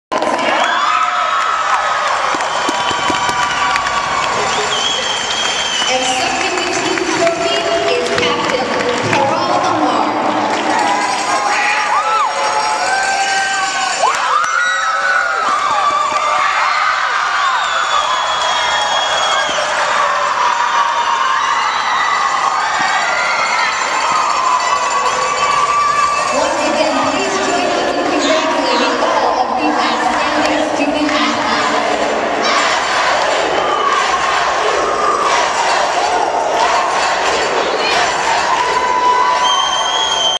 Saints Presented ECAC Championship Trophy
Karell Emard, Josee Belanger and the St. Lawrence University women's hockey team are presented the Championship trophy after winning the ECAC Tournament title for the first time in program history.